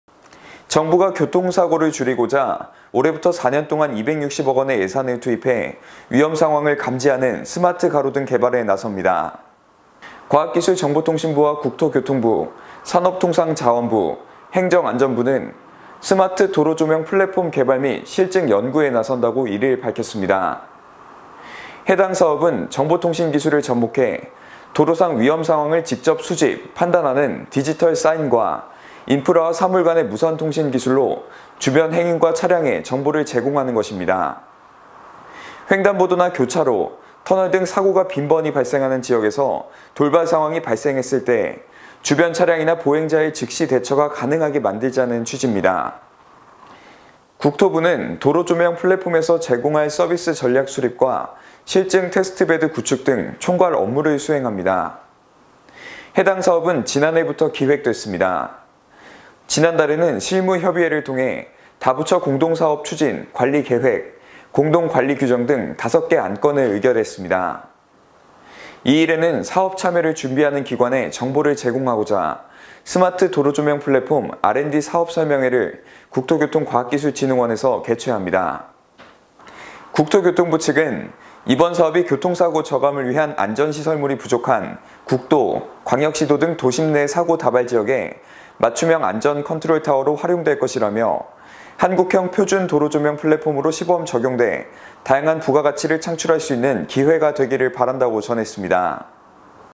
[라이브썰] 정부, 교통사고 저감 위한 ‘스마트 가로등’ 개발 착수
정부가 교통사고를 줄이고자 올해부터 4년 동안 260억 원의 예산을 투입해 위험상황 감지하는 스마트 가로등 개발에 나섭니다.

과학기술정보통신부와 국토교통부, 산업통상자원부, 행정안전부는 ‘스마트 도로조명 플랫폼 개발 및 실증연구’에 나선다고 1일 밝혔습니다.

해당 사업은 정보통신기술(ICT)을 접목해 도로상 위험상황을 직접 수집·판단하는 ‘디지털 사인’과 인프라와 사물간의 무선통신(I2X) 기술로 주변 행인과 차량에 정보를 제공하는 것입니다. 횡단보도나 교차로, 터널 등 사고가 빈번히 발생하는 지역에서 돌발 상황이 발생했을 때 주변 차량이나 보행자의 즉시 대처가 가능하게 만들자는 취지입니다.

국토부는 도로조명 플랫폼에서 제공할 서비스 전략수립과 실증 테스트베드 구축 등 총괄업무를 수행합니다.

해당 사업은 지난해부터 기획됐습니다. 지난달에는 실무협의회를 통해 다부처 공동사업 추진·관리계획, 공동관리규정 등 5개 안건을 의결했습니다.

2일에는 사업 참여를 준비하는 기관에 정보를 제공하고자 스마트 도로조명 플랫폼 R&D 사업설명회를 국토교통과학기술진흥원에서 개최합니다.

국토교통부 측은 “이번 사업이 교통사고 저감을 위한 안전 시설물이 부족한 국도·광역시도 등 도심 내 사고 다발지역에 맞춤형 안전 컨트롤 타워로 활용될 것”이라며 “한국형 표준 도로조명 플랫폼으로 시범 적용돼 다양한 부가가치를 창출할 수 있는 기회가 되기를 바란다”고 전했습니다.